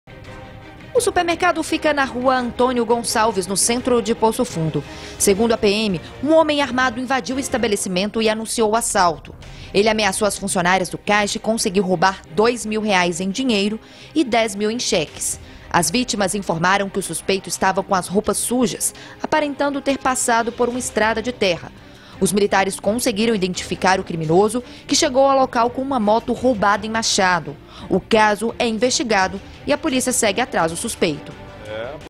Bandido assalta supermercado em Poço Fundo
SUPERMERCADO É ASSALTADO EM POÇO FUNDO | Um supermercado foi alvo de bandido armado em Poço Fundo.